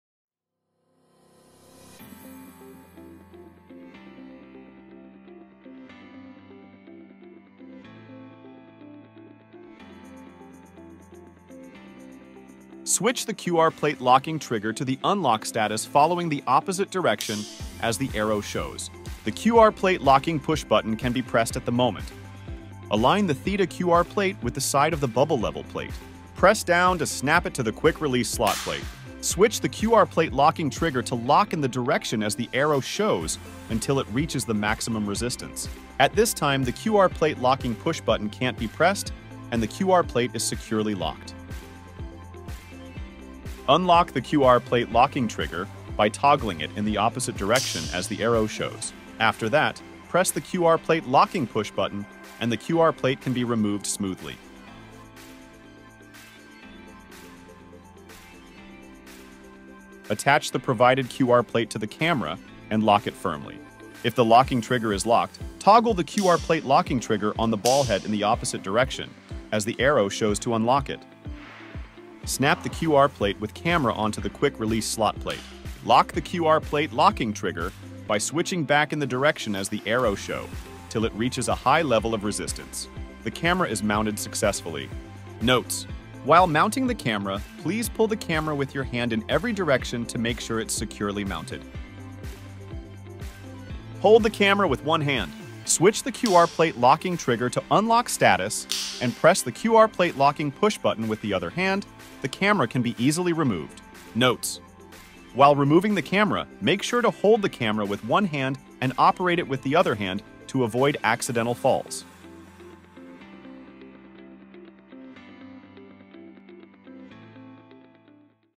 Installment & Removal of the QR Plate & Camera on Benro Theta & Theta Max
Check how to install & remove the QR Plate and camera on Benro Theta & Theta Max.

Chapters:
How to install the QR plate onto the tripod: 00:12
How to remove the QR plate from the tripod: 00:43
How to mount the camera into the tripod: 00:59
How to remove the camera from the tripod 01:32